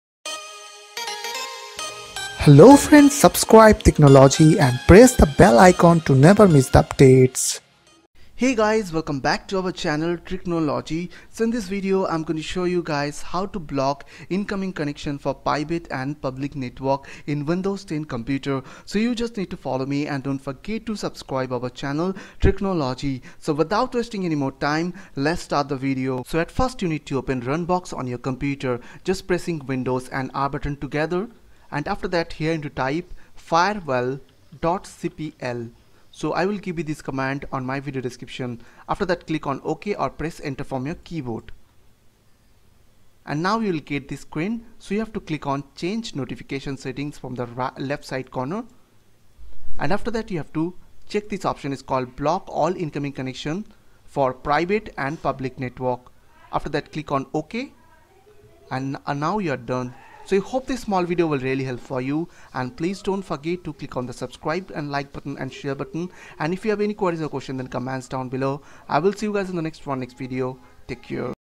How To Block Incoming Connections For Private and Public Network In Windows 10
In this video I will show you guys how to Block Incoming Connections For Private and Public Network In Windows 10 pc.

Run command : firewall.cpl

1. Mic
2. Microphone Stand
3. Headphone
4. Phone
5. Strip LED Light